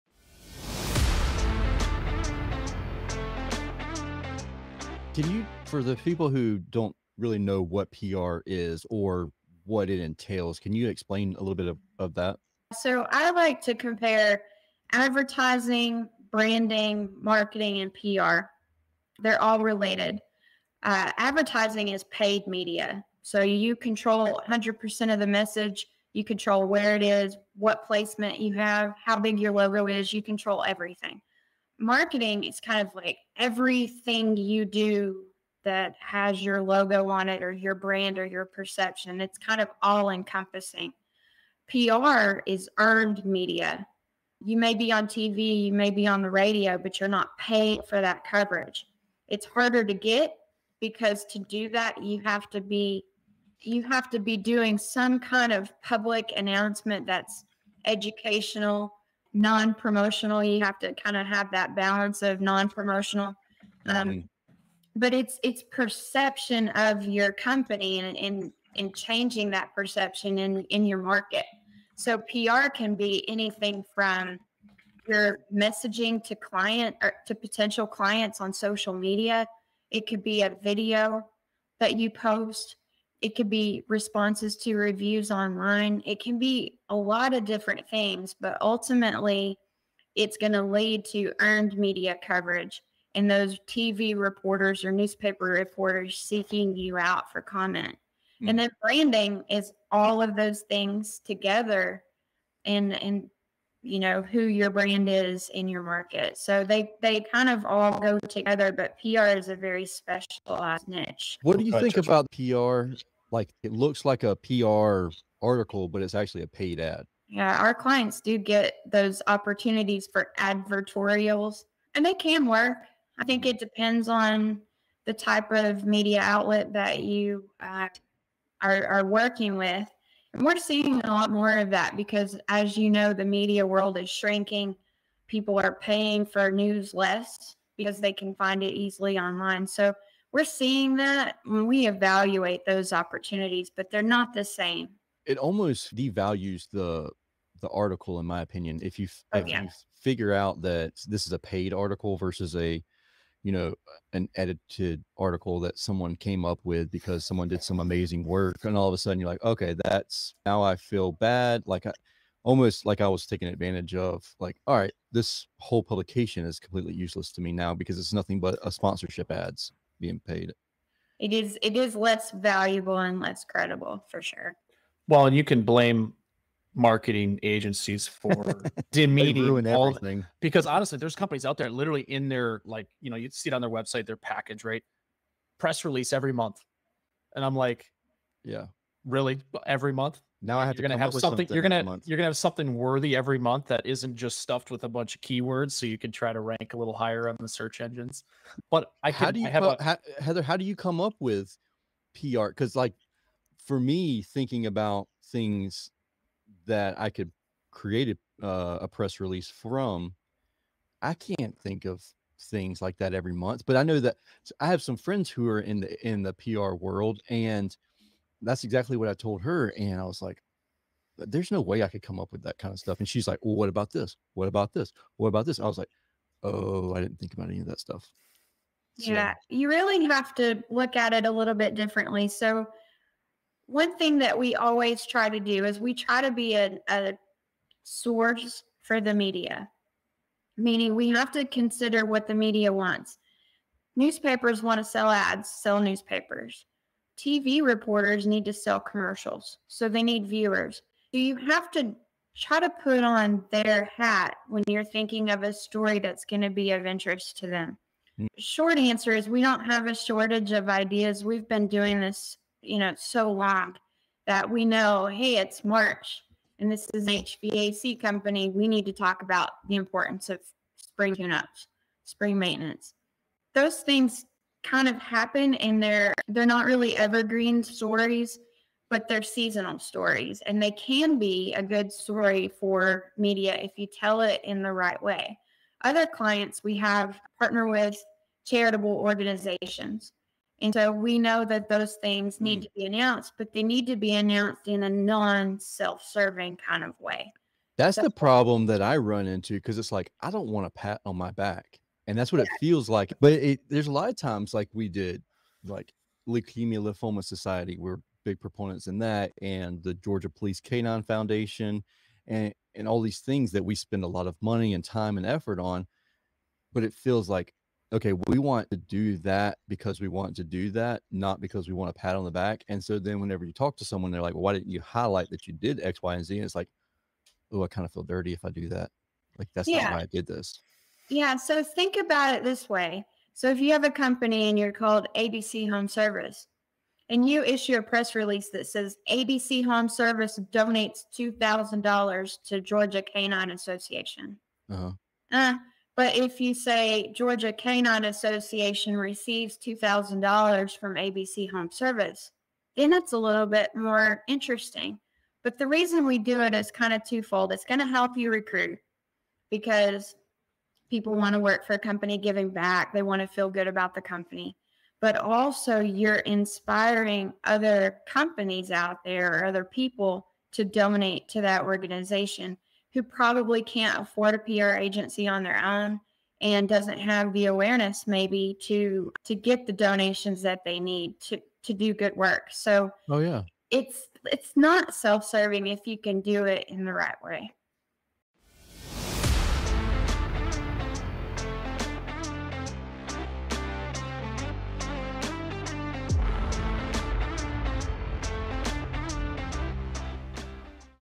PR 101: How to Implement an Effective PR Strategy for Your Home Service Business
Why should contractors care about public relations? To scrape the surface, PR can help you get new customers, reach your business goals, create goodwill in your local community, and even help you sell when you want to retire! So, why wait until a major accident happens to develop a relationship with a PR firm? Let’s dive into it!

Meet Heather Ripley (LinkedIn), CEO and Founder of RipleyPR & Author of "Next Level Now, PR Secrets to Drive Explosive Growth for Your Home Service Business".

In this episode we discuss:
The difference between public relations, advertising, branding, and marketing.
When and how to create and scale new PR opportunities for your contracting company.
Implementing an effective PR strategy based on your business goals.

Buy Heather Ripley’s book, Next Level Now.

Listen on Amazon Music